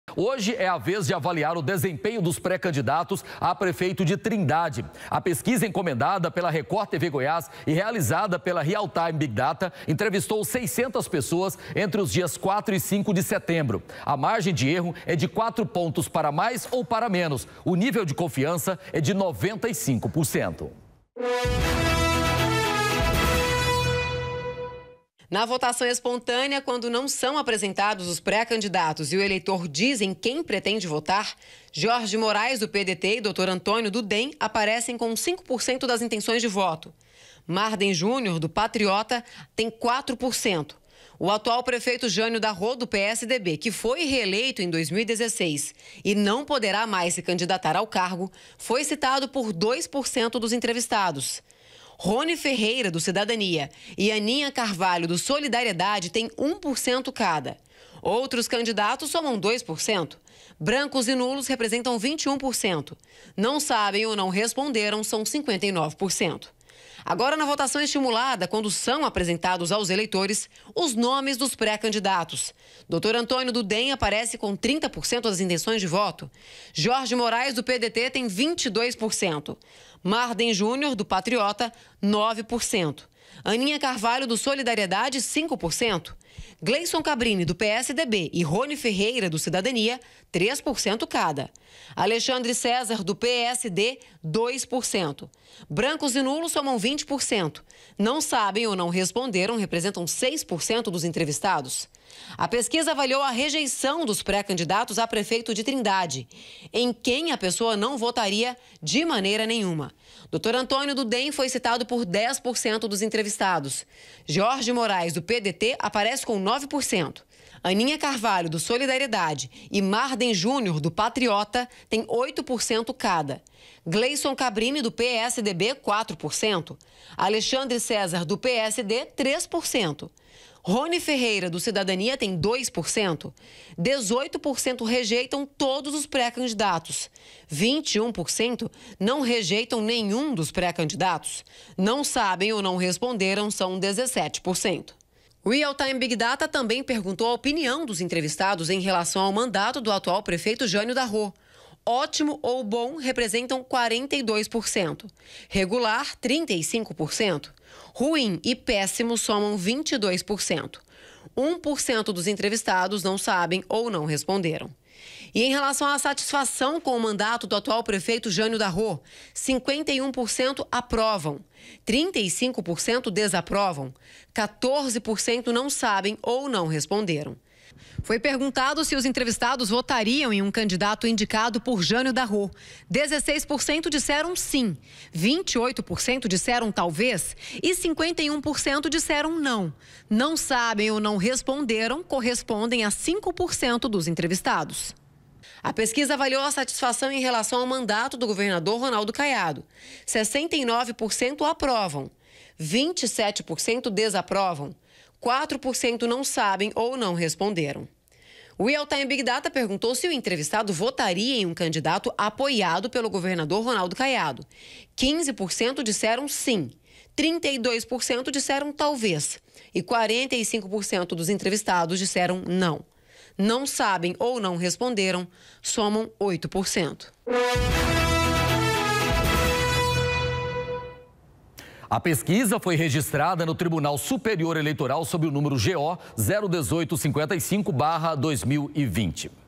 PESQUISA ELEITORAL - TRINDADE
Hoje é a vez de avaliar o desempenho dos pré-candidatos a prefeito de Trindade. A pesquisa encomendada pela Record Tv Goiás e realizada pela RealTime Big Data entrevistou 600 pessoas entre os dias 4 e 5 de setembro. A margem de erro é de 4 pontos para mais ou para menos e o nível de confiança é de 95%.
PESQUISA REGISTRADA NO TSE SOB O Nº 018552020